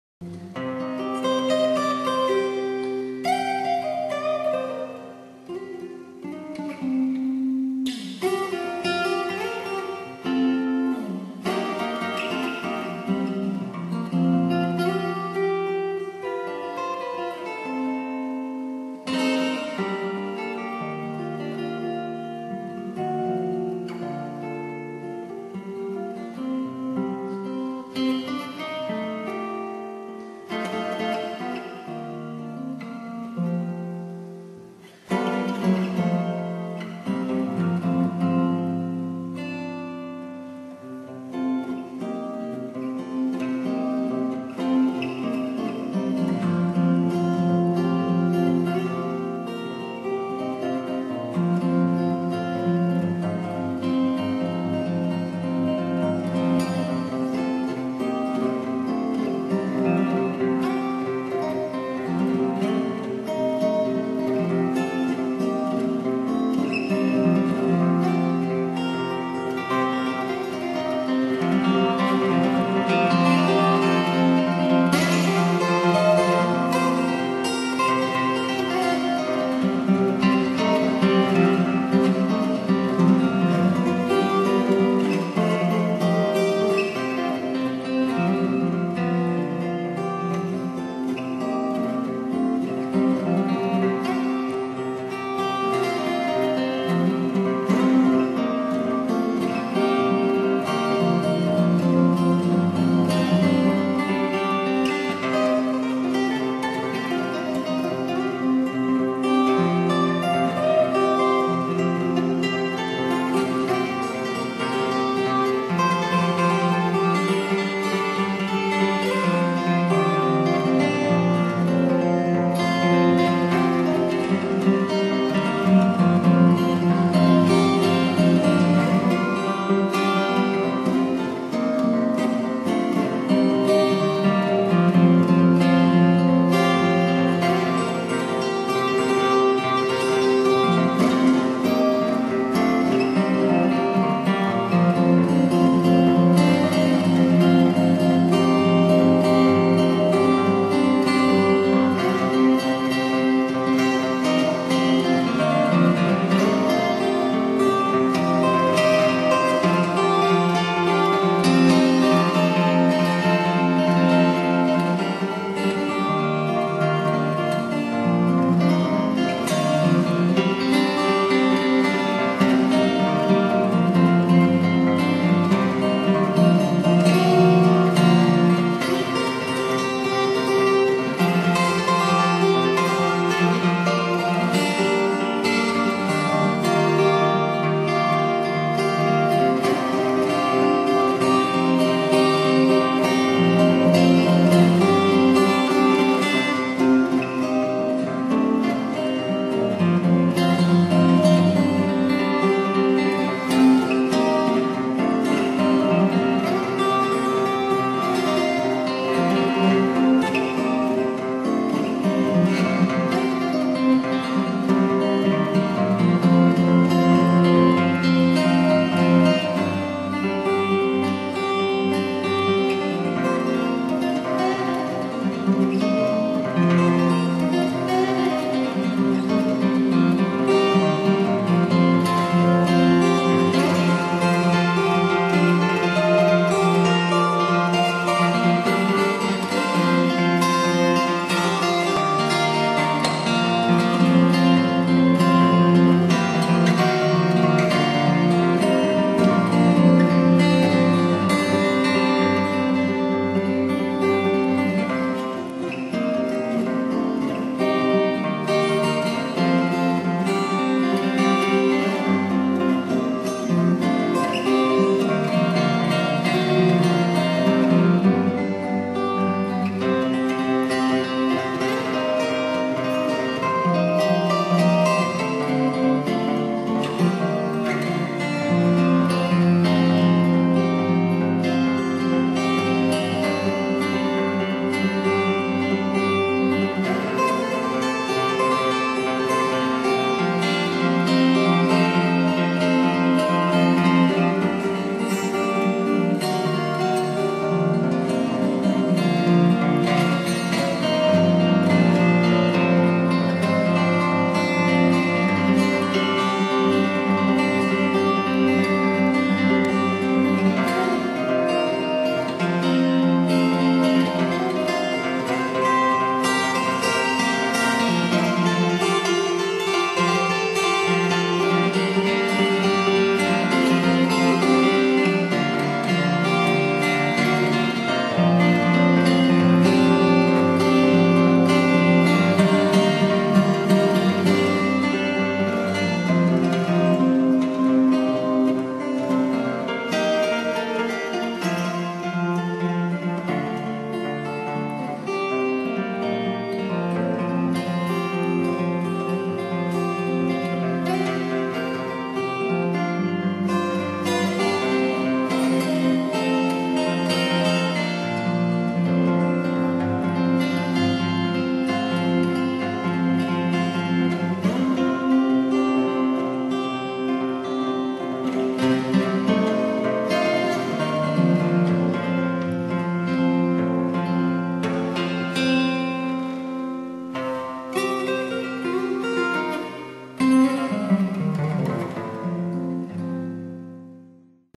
small train in china
imaginary trainride on accoustic guitar